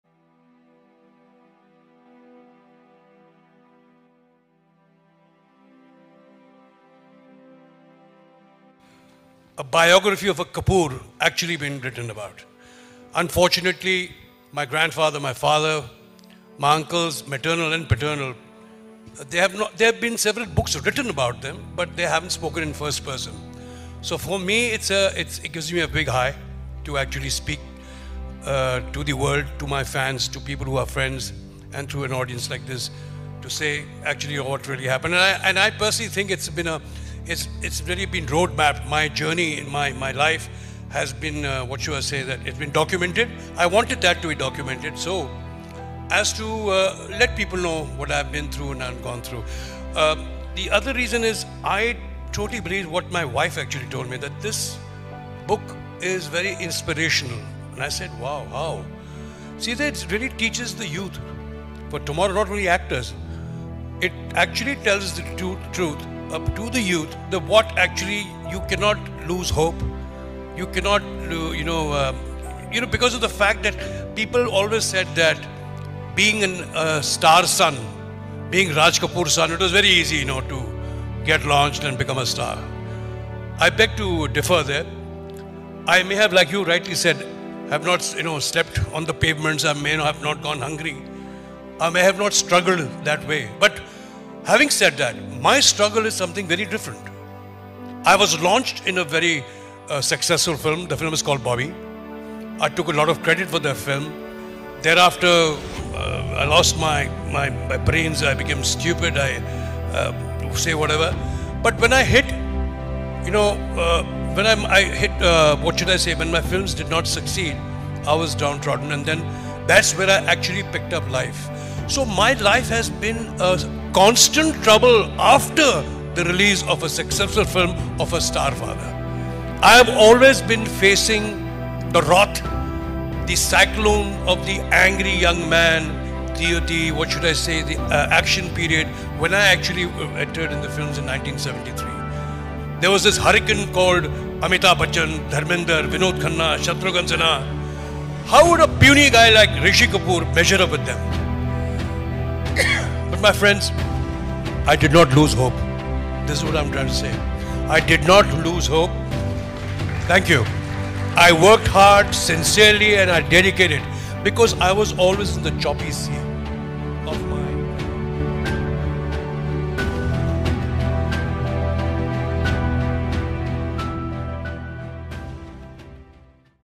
The Untold Story Of My Life By Rishi Kapoor | 1952 - 2020 | Motivation Mantra
Rishi Kapoor was an Indian actor known for his work in Hindi cinema. He received the National Film Award for Best Child Artist for his debut role in his father Raj Kapoor's 1970 film Mera Naam Joker.

Special Tribute To Rishi Kapoor Sir

Motivation Mantra is Non-Profit Channel.

Speaker : Late Shri Rishi Kapoor( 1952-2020 )

||     Keep Supporting Our team          ||
||         MOTIVATION MANTRA            ||